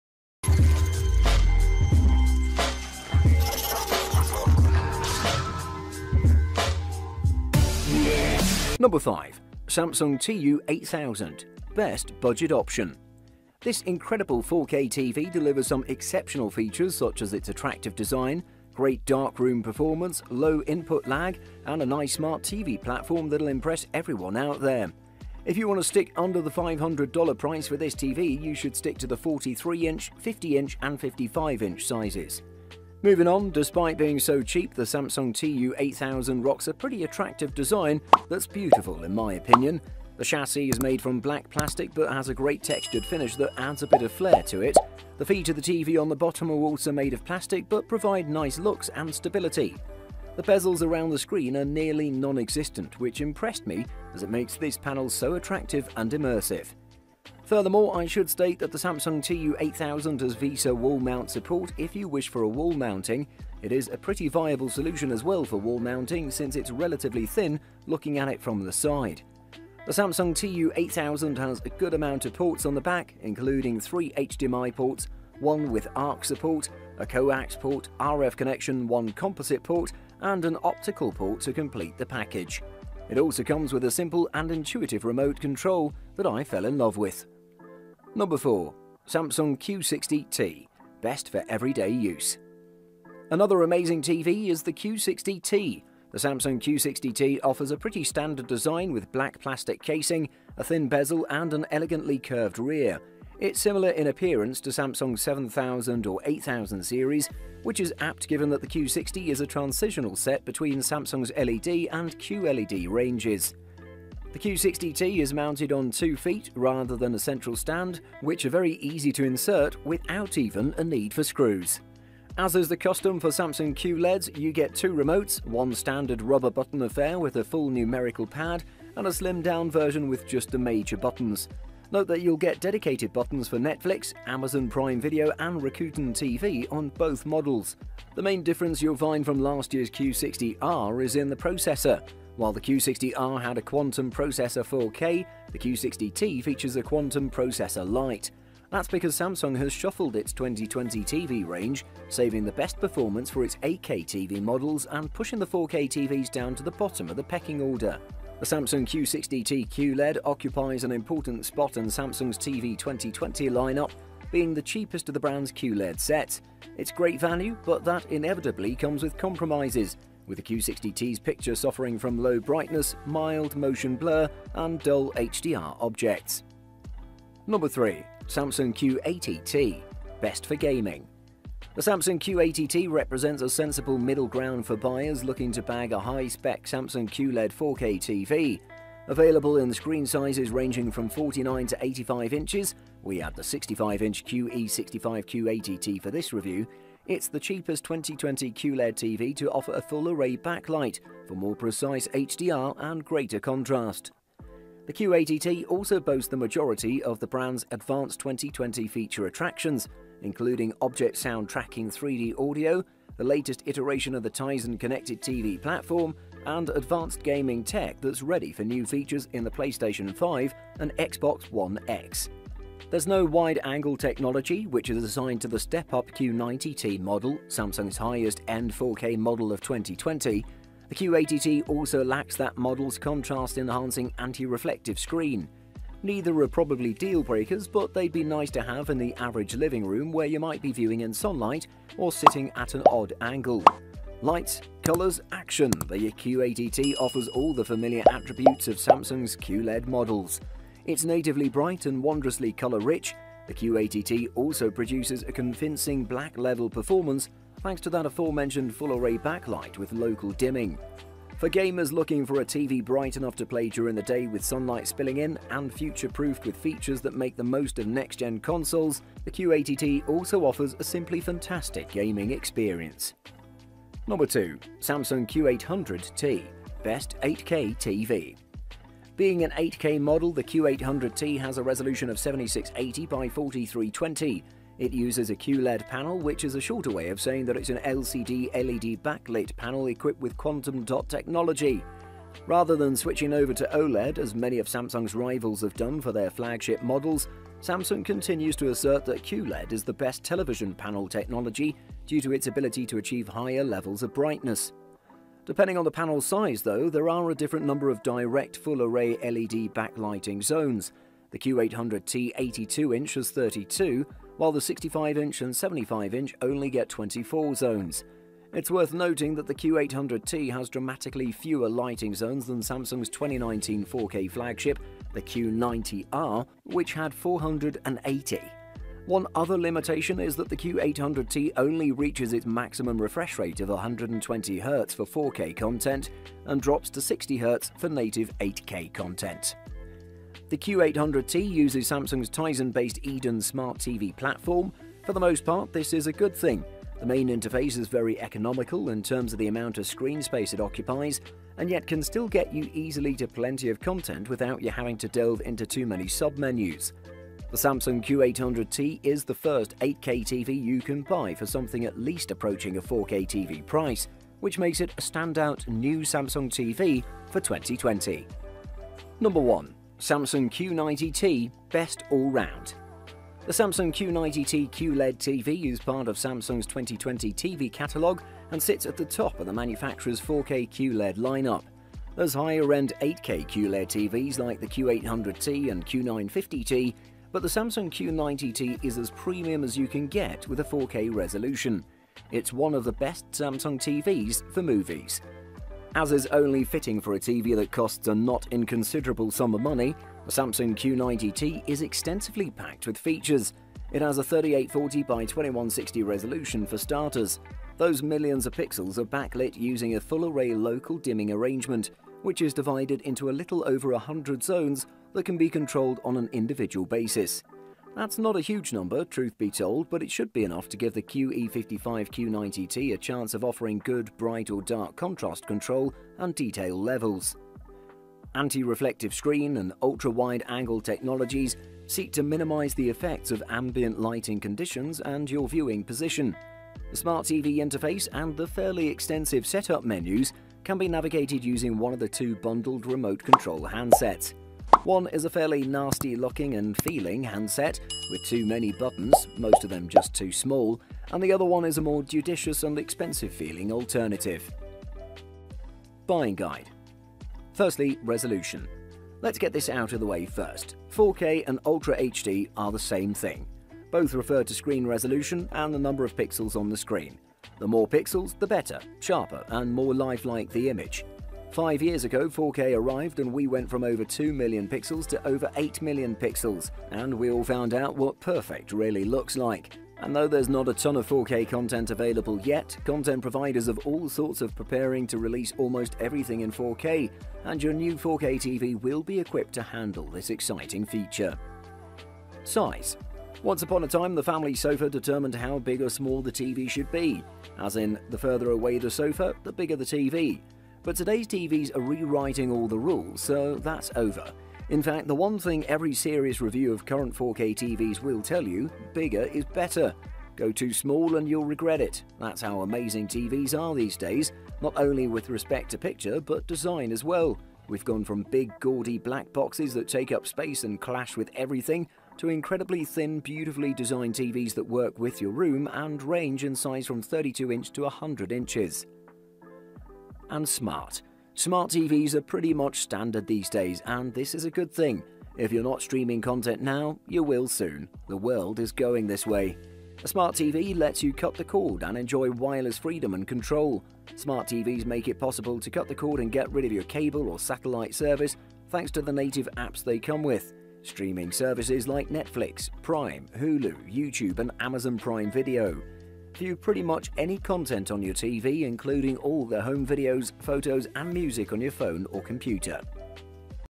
✅ 2022 Review Samsung 4K Q60T QLED TV | Top 5 Best Samsung TVs 2022
► Links to the best samsung tvs 2022 we listed in this video:

► 5. Samsung TU8000
► 4. Samsung Q60T
► 3. Samsung Q80T
► 2. Samsung Q800T
► 1. Samsung Q90T